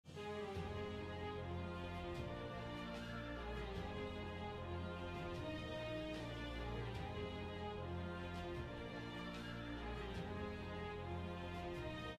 Scorpion’s new SADDLE-CAST!
His tail has been broken for so long, so I really hope this cast lasts longer than previous things I’ve tried.